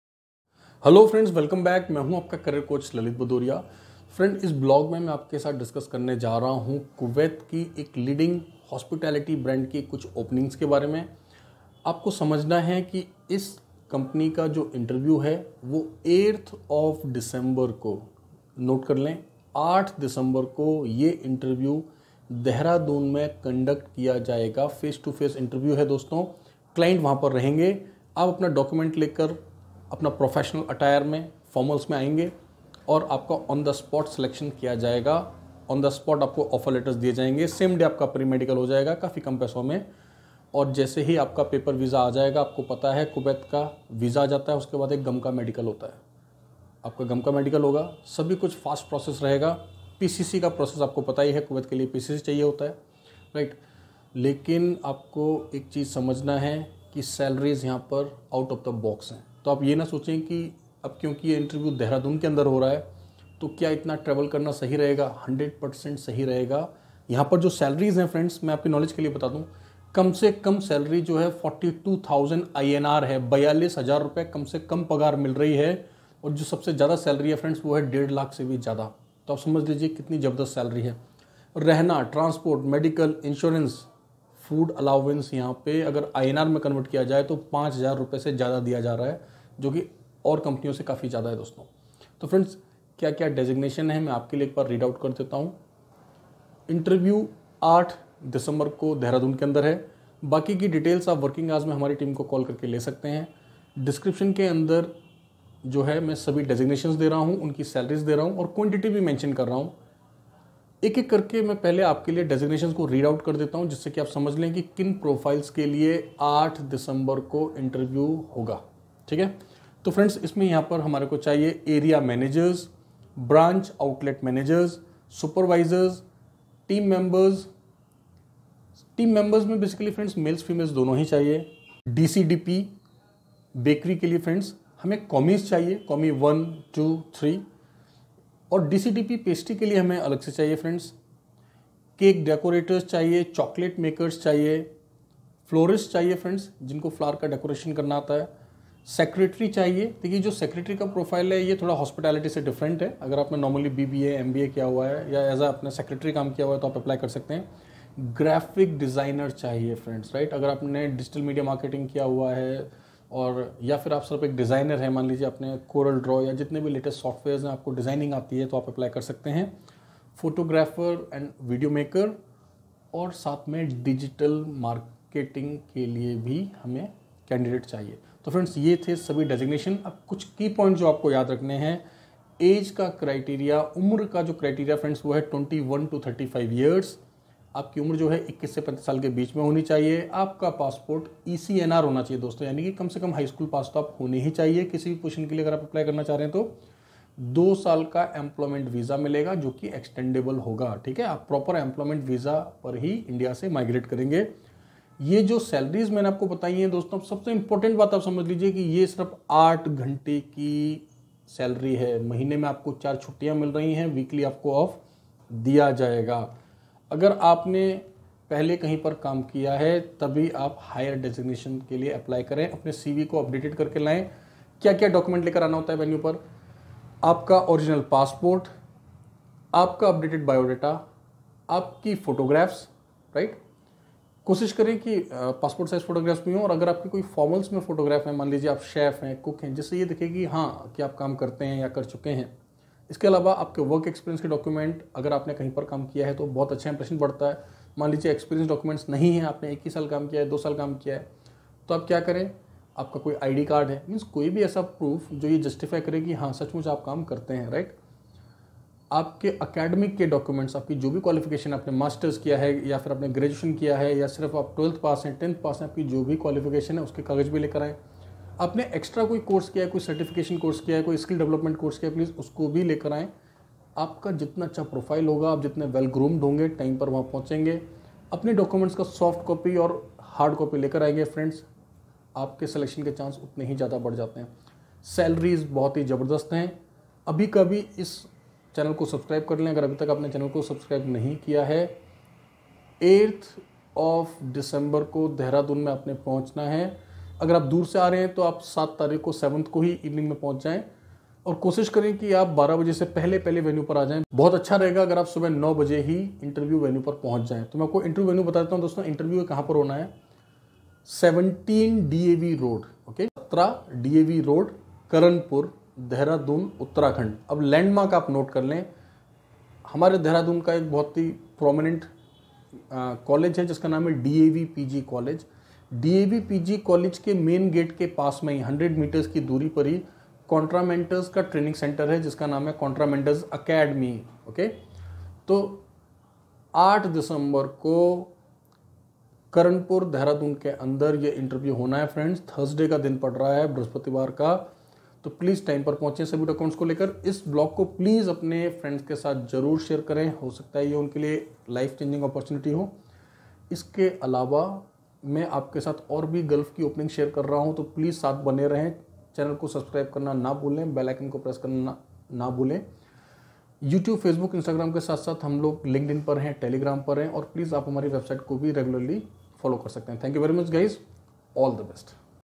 Kuwait Employment Visa | Kuwait Client Interview in Dehradun
Client Interview for Kuwait Leading Hospitality Brand in Dehradun on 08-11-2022(Thursday)

 ACCOMMODATION + TRANSPORTATION PROVIDED BY COMPANY
 DAILY WORKING HOURS 8 HOURS PER DAY, 6 DAYS A WEEK
 CONTRACT PERIOD 2 YEARS
 ECR / ECNR  ECNR ONLY
 EXPERIENCE  PREVIOUS EXPERIENCE IN FAST FOOD CHAIN OR HOTEL
 LANGUAGE SKILLS GOOD KNOWLEDGE IN ENGLISH COMMUNICATION AND WRITING
 AGE LIMIT 21-35 YRS

Sr. No Positions Required Quantity Basic Salary Food Allowance Total Salary Salary INR  Food Allowance (Total Salary )
S.no Positions Required Basic Salary in KD Food Allowance in KD Total Salary in KD
1 Area Manager                      KD 400 – 600 - KD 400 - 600
2 Branch/Outlet Manager       KD 280     KD 20      KD 300
3 Supervisor                                KD 200      KD 20 KD 220
4 Team Member / Sandwich-FEMALE KD 140 KD 20 KD 160
5 Team Member / Sandwich- MALE KD 140 KD 20 KD 160
6 DCDP (bakery) KD 200 KD 20 KD 220
7 Commi I (bakery) KD 180 KD 20 KD 200
8 Commi II (bakery)KD 160 KD 20 KD 180
9 Commi III (bakery)KD 130 KD 20 KD 150
10 DCDP (pastry) KD 200 KD 20 KD 220
11 Commi I (Pastry) KD 180 KD 20 KD 200
12 Commi II (pastry) KD 160 KD 20 KD 180
13 Commi III (pastry)KD 130 KD 20 KD 150
14 Cake Decorator KD 180 KD 20 KD 200
15 Chocolate Maker KD 160 KD 20 KD 180
16 Florist (Flower Decorator) KD 140 KD 20 KD 160
17 Secretary - Female KD 200 – 220 - KD 200 – 220
18 Graphics Designer - Male / Female KD 300 – 450 -KD 300 – 450
19 Photographer / Video maker KD 300 – 450 -KD 300 – 450
20 Digital Marketing                   KD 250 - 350 - KD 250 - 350